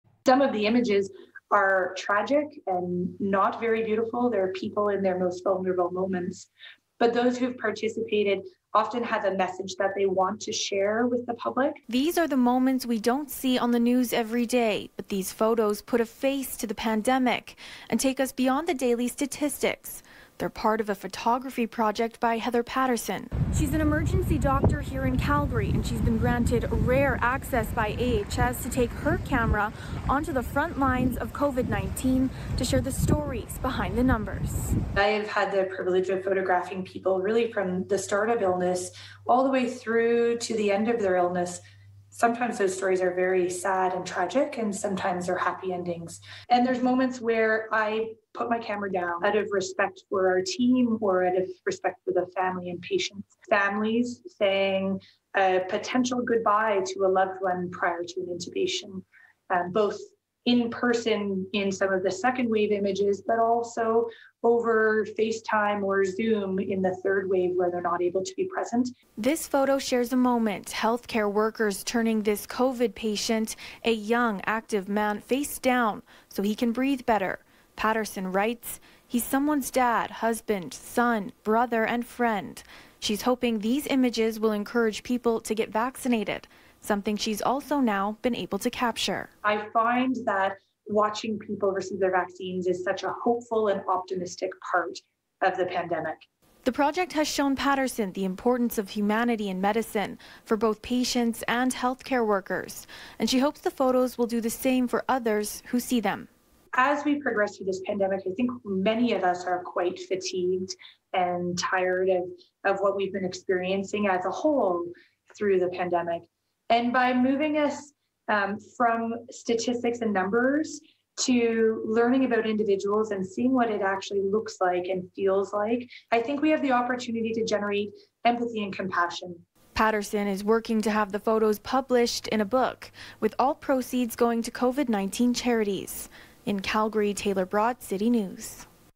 Rare photographs reflect fight against COVID-19
A Calgary doctor has been granted access to the rooms of COVID-19 patients to take photos. As Taylor Braat reports, the doctor wants to inspire more empathy in the pandemic.